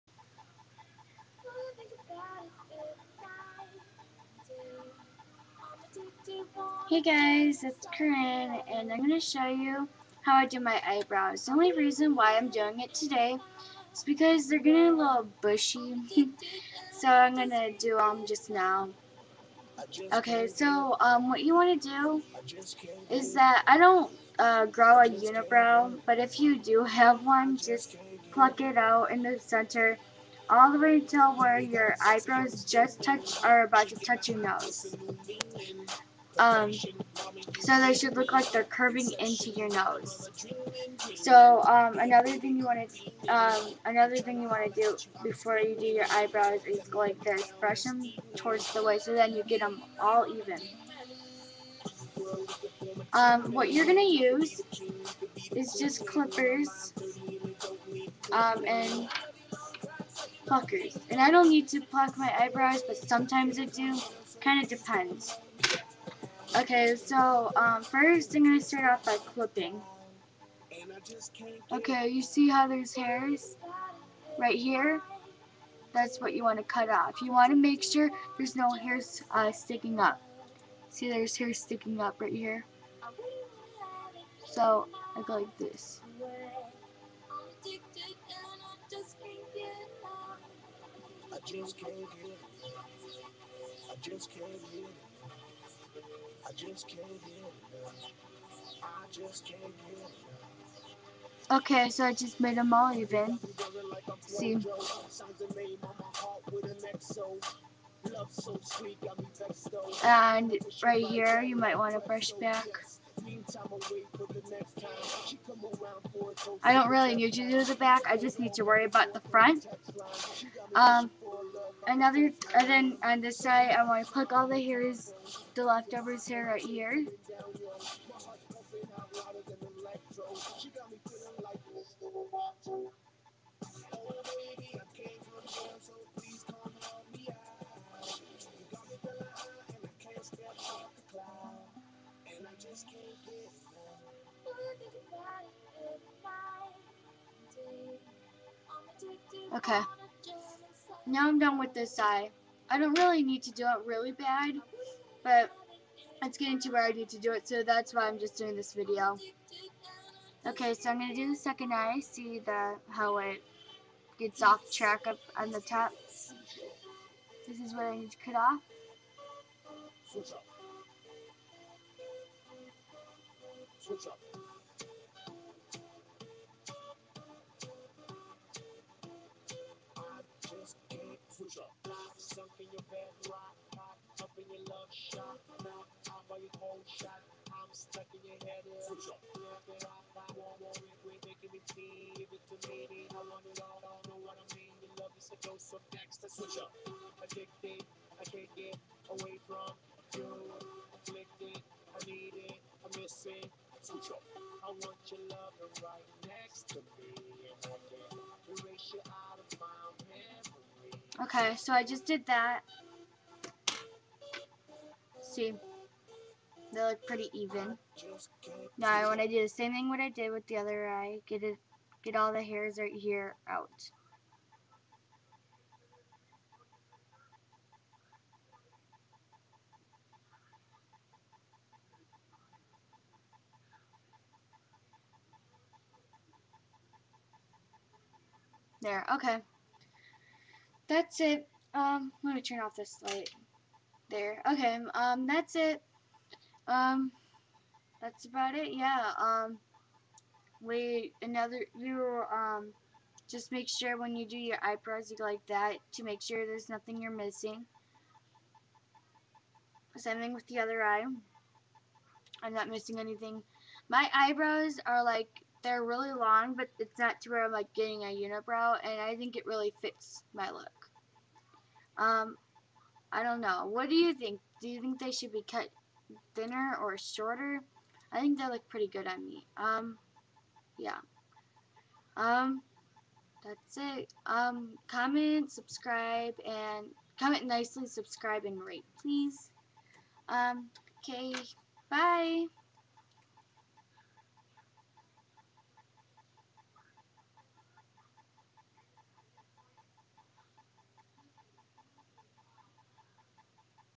Corynn: How I Do My Eyebrows
This is how I do my eyebrows. Brooke would do a video but her eyebrows don't grow long...LOL. But yeah Enjoy